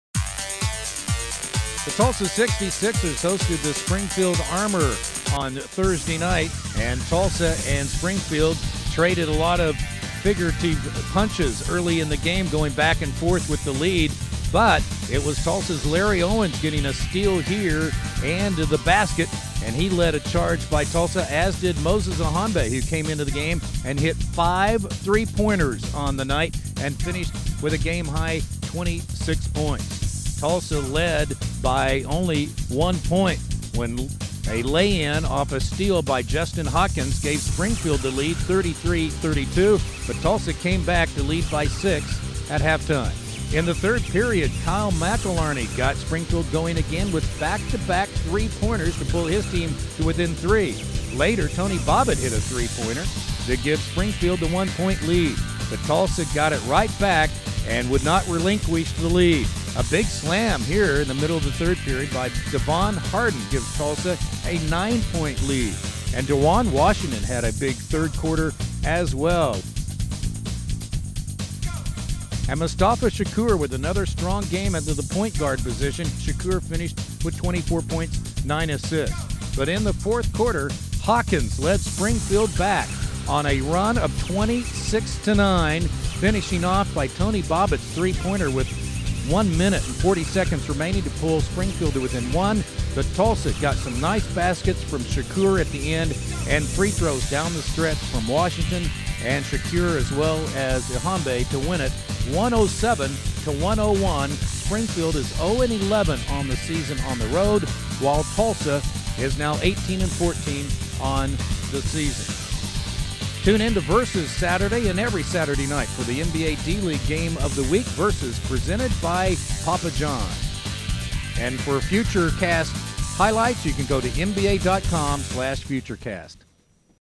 Armor 101, 66ers 107
Tulsa 66ers come away with a hard fought victory over the Springfield Armor with a score of 107 to 101.